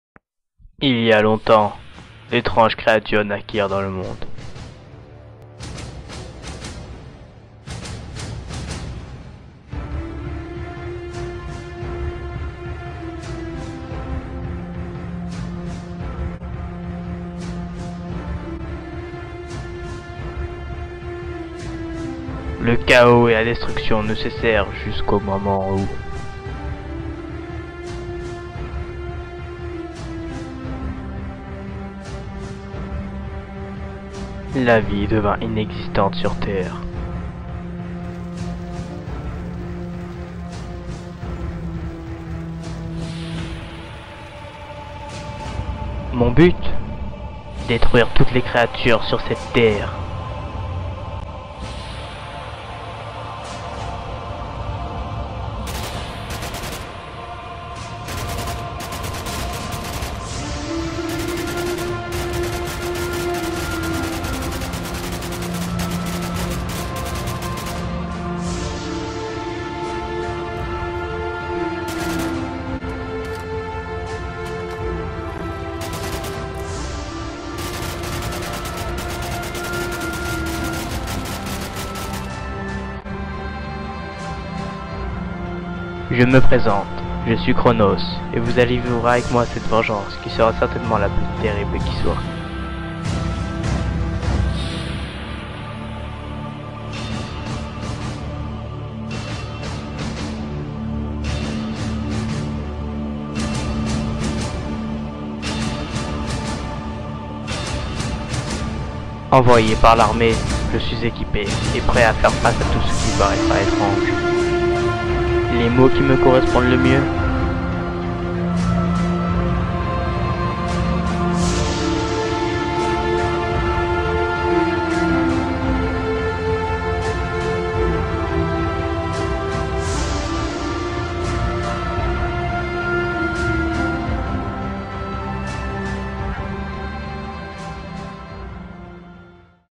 [TRAILER]Cronos Or Die
Le trailer de la future série qui sortira après celle de Lost Les Youtubeurd Disparus est désormais disponible ! :)

J'espère qu'il vous plaira, bye !

Bon visionnage